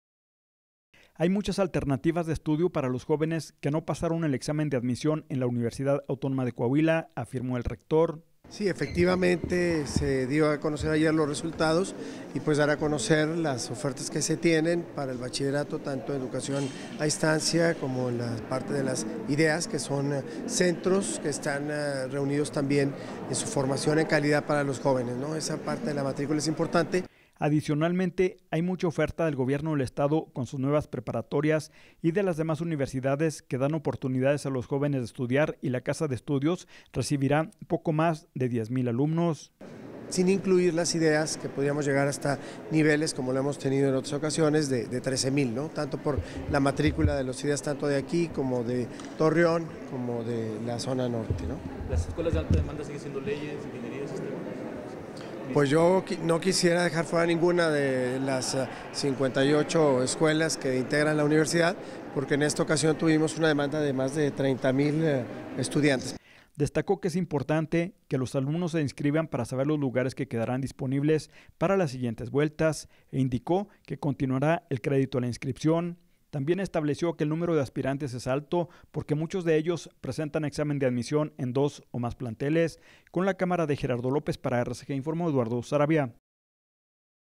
Sí hay oportunidades de estudio para alumnos que no pasaron examen en UAdeC
Hay muchas alternativas de estudio para los jóvenes que no pasaron el examen de admonición en la Universidad Autónoma de Coahuila, afirmó el rector.